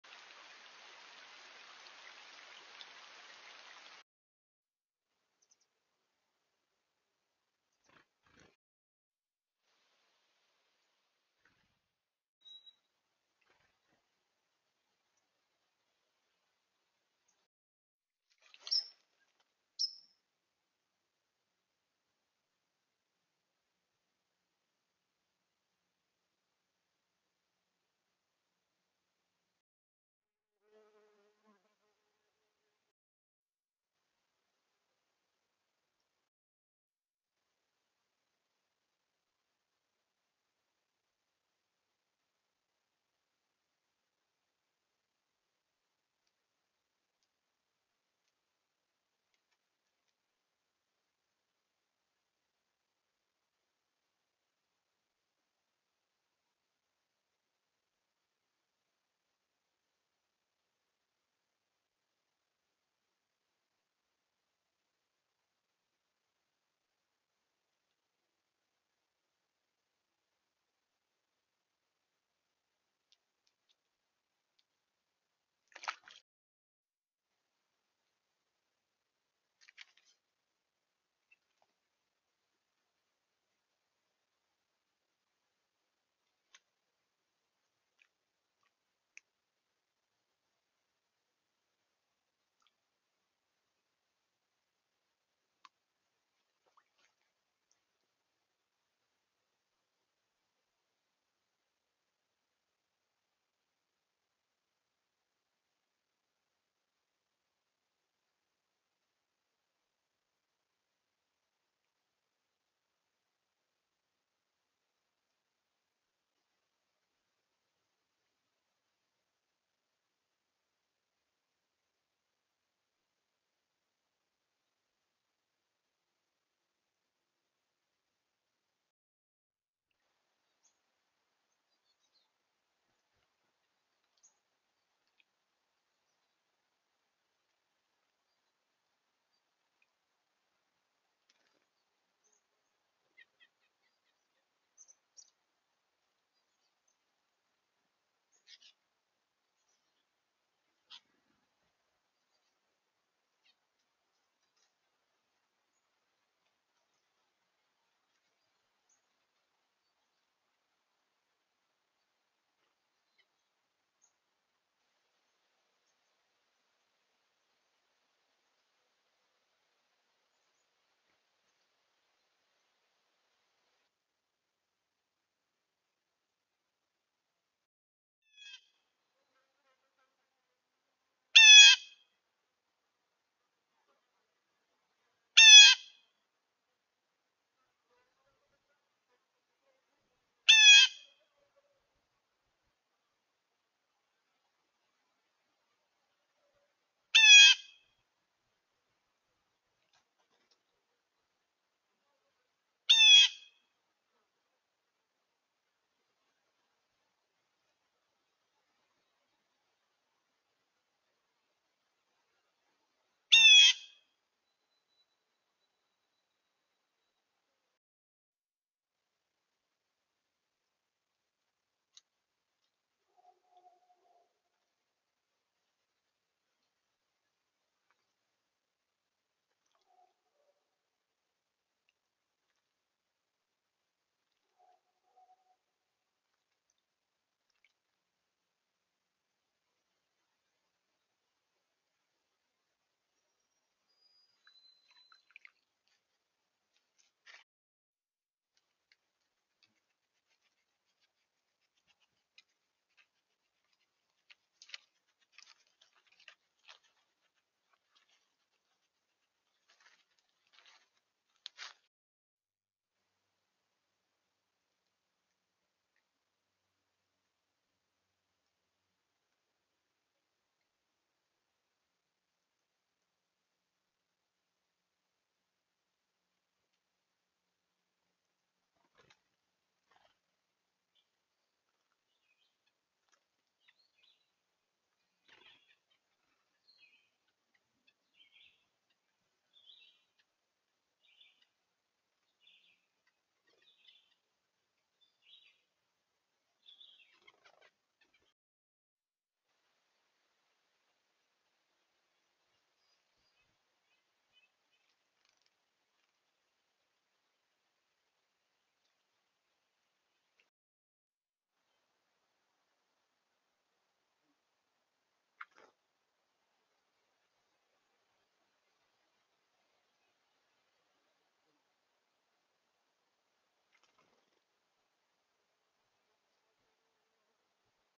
BirdBathCam2 Movie 3
We named this camera BirdBathCam after setting it up beside a small pool that several birds were using for bathing.

Cast at time of first appearance:
0:00 White-throated Sparrow
0:04 American Robin
0:17 Marten
0:20 ? Spider
0:35 ? Meadowhawk
0:37 Broad-winged Hawk
2:09 Red-breasted Nuthatch
2:58 Blue Jay
4:01 ? Jumping Mouse
4:07 Striped Skunk
4:59 Yellow-rumped Warbler